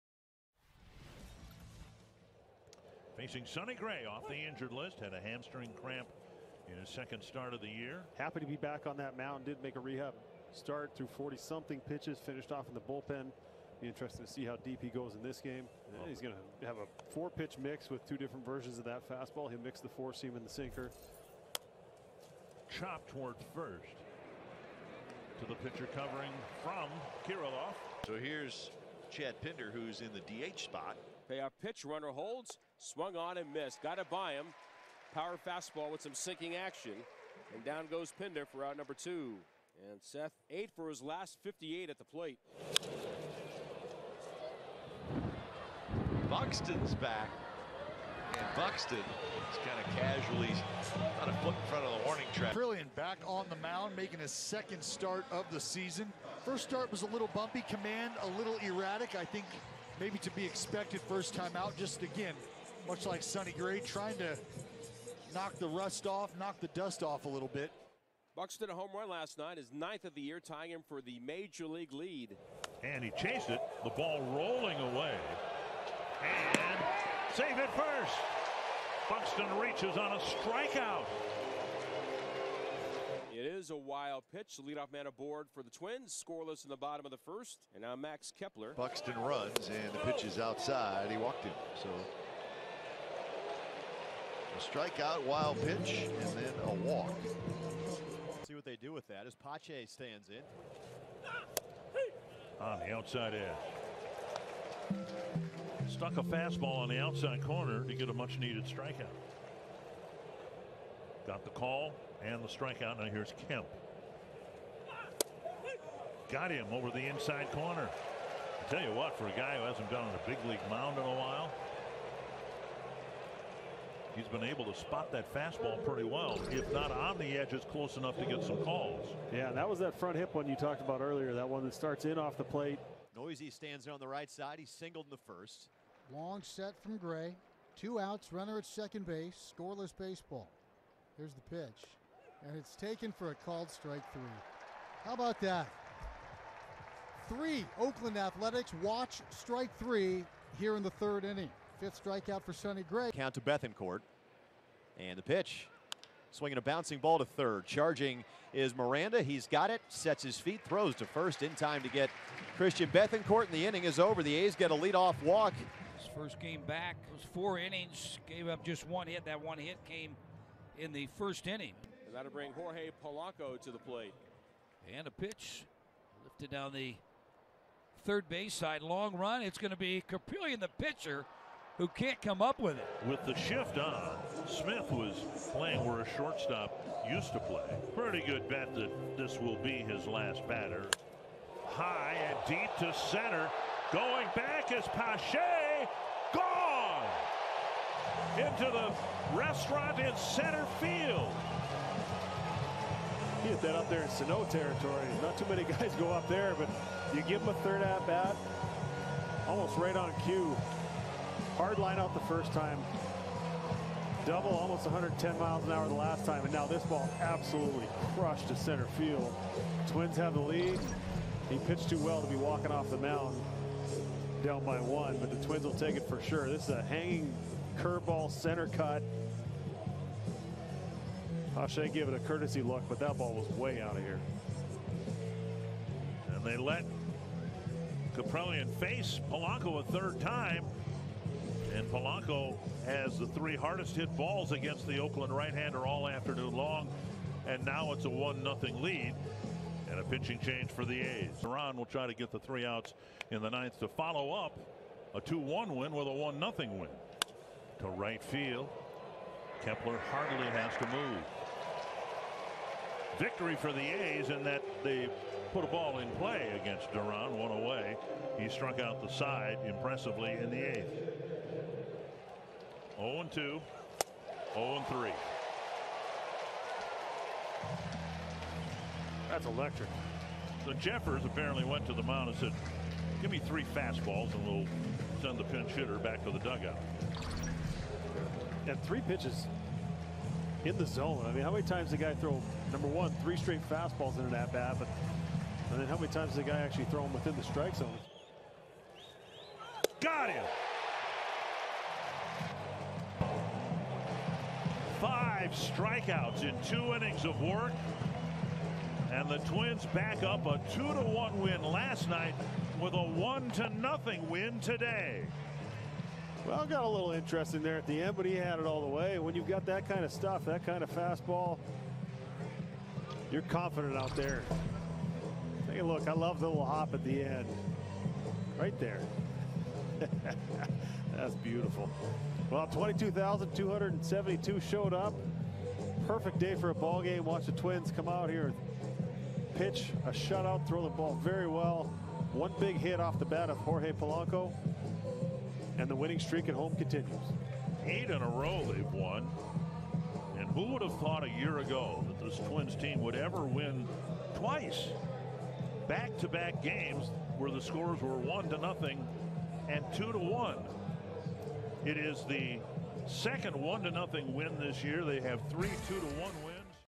Minnesota Twins Highlights vs Oakland Athletics. 4/7/22
Let's Go 1 - 0 Win. The Twins Are Looking ALOT  Better Than last Year.

All Clips And Audio Belong To The MLB. Major League Baseball. Bally Sports. NBC Sports. Bally Sports North. NBC Sports California.